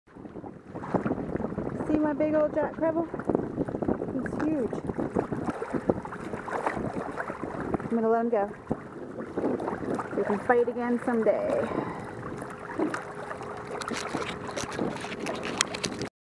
Crevalle Jack, lots of fun to catch, taste like crap
Kayak Girl caught this jack, its the biggest one she's caught to date.